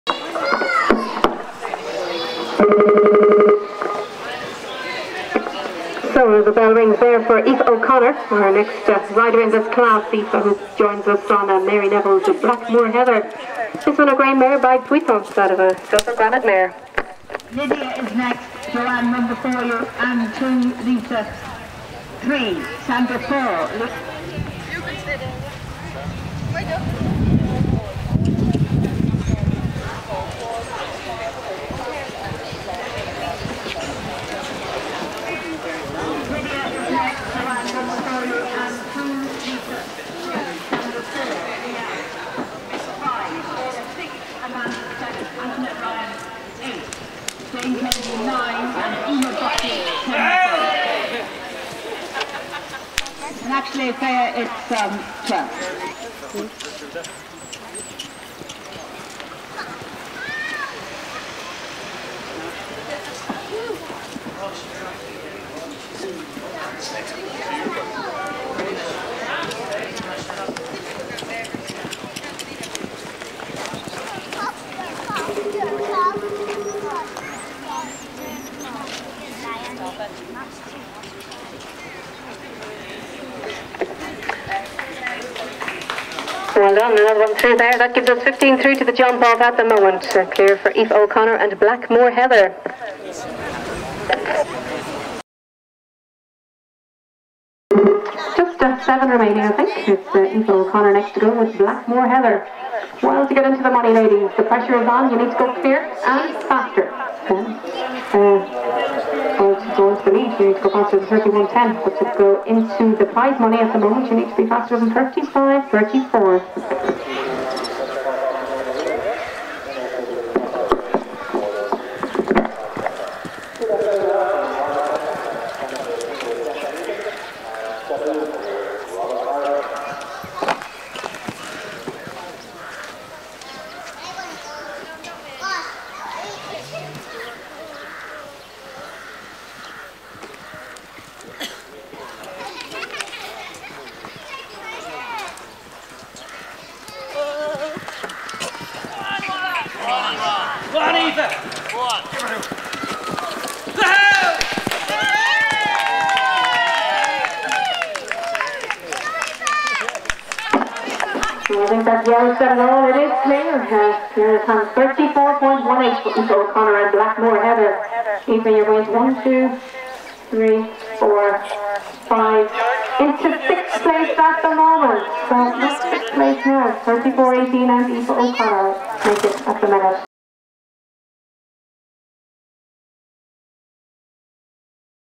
Heather come 6th in Ballymaloe Leinster Final
Blackmoor Heather ridden by Aoife O'Connor comes 6th in the Ballymaloe Leinster Final